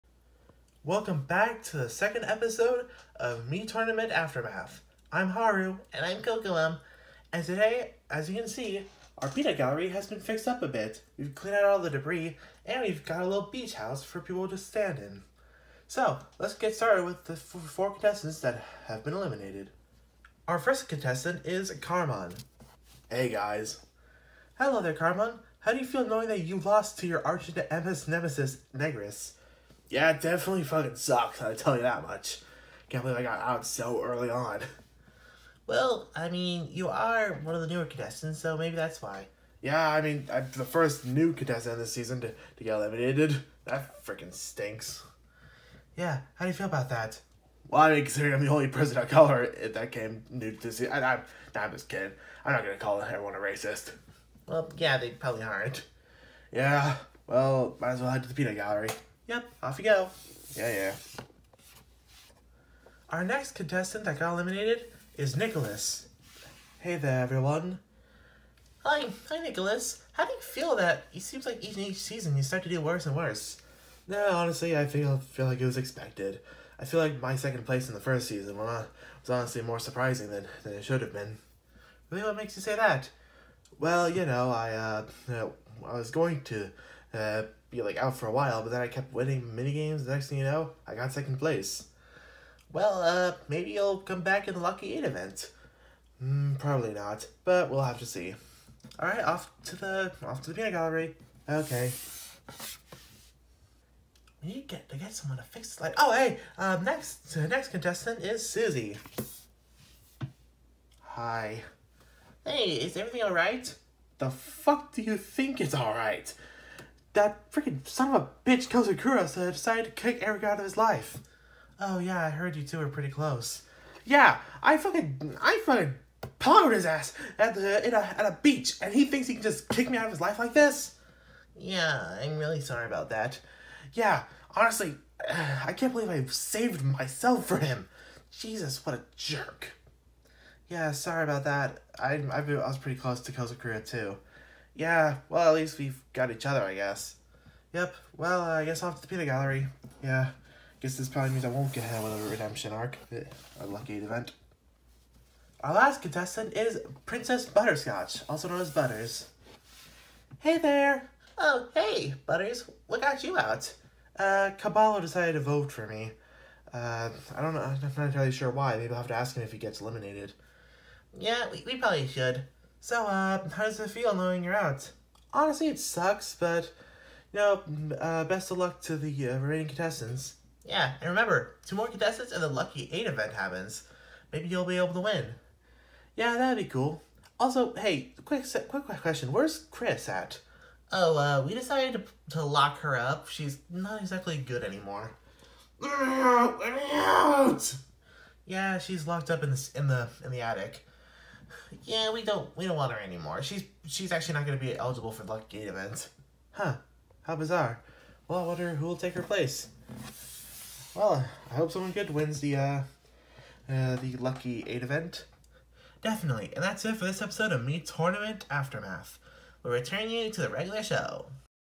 Mii Tournament Trifecta: Episode 8C: Aftermath 2
In this episode, Haru and Cocoam talk to 4 more contestants that have been eliminated. Carmon, Nicolas, Susie, and Butters. Also, the aftermath set has been cleaned a lot since you last saw it. No more sitting on rubble!